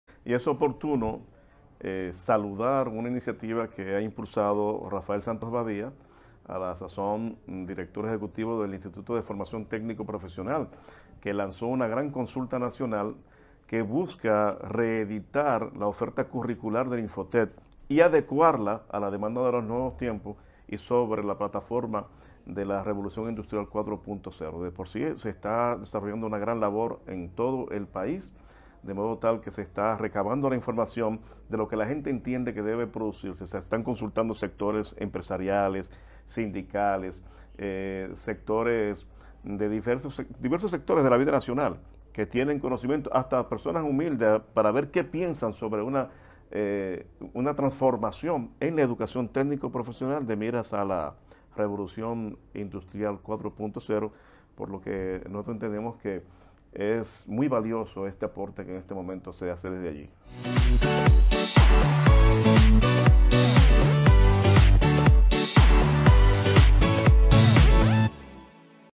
Edwin DeLaCruz comenta iniciativa de Rafael Santos Badía con la Consulta Nacional
La Consulta Nacional sobre el Futuro de la Formación Técnico Profesional en la República Dominicana es un mecanismo de participación, de búsqueda de consensos, de promover compromisos, de lograr identificación sobre el futuro del empleo en el país y su incidencia en el accionar del INFOTEP, para responder de manera efectiva y adelantada, a dichos cambios.